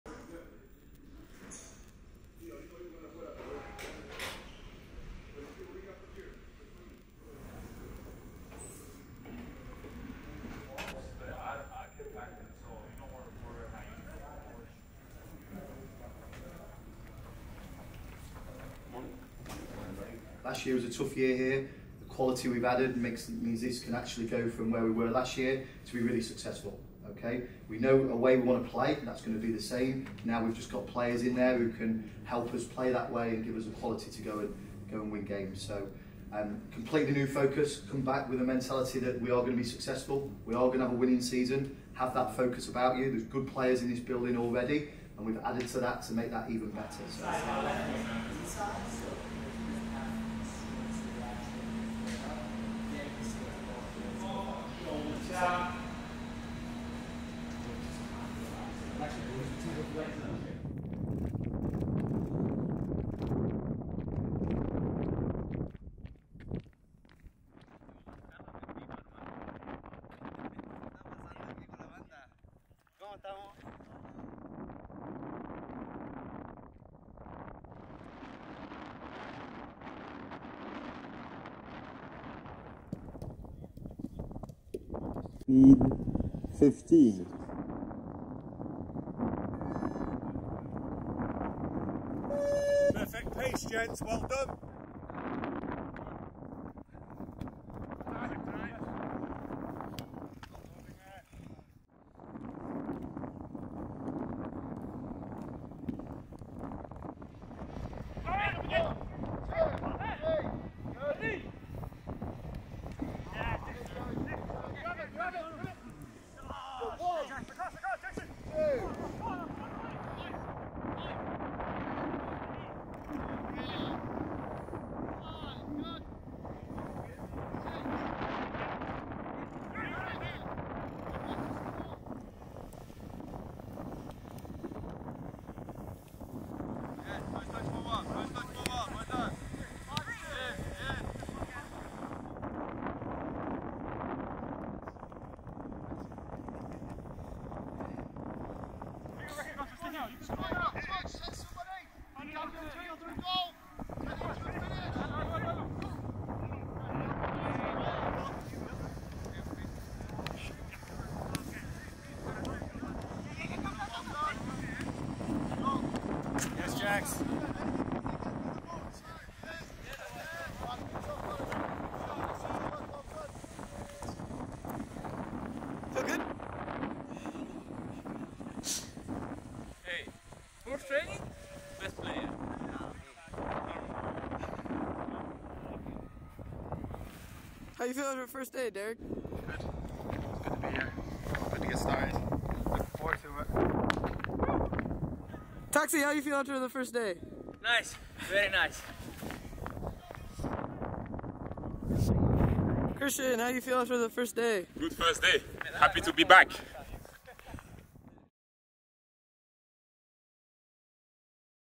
👀 Inside Day 1 of Preseason | The Black-and-Red Begin Preparation for the 2023 MLS Season
Take in all of the sights and sounds from the United Performance as the Black-and-Red report for the start of the 2023 preseason.

For all things D.C. United » dcunited.com